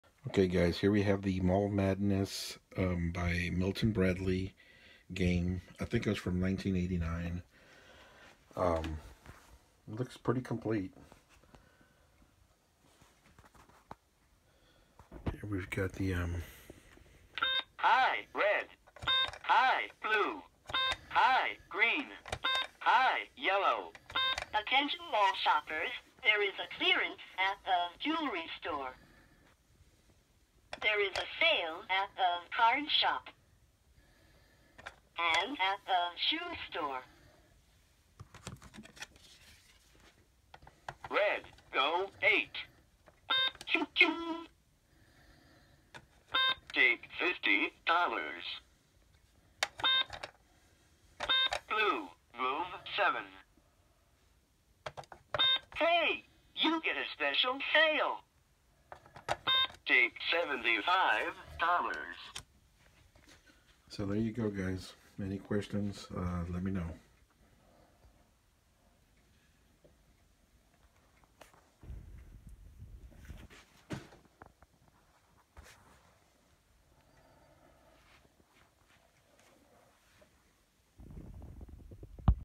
VINTAGE 1989 MILTON BRADLEY MALL MADNESS BOARD GAME
VINTAGE 1989 MILTON BRADLEY MALL MADNESS BOARD GAME, COMPLETE AND WORKS PERFECTLY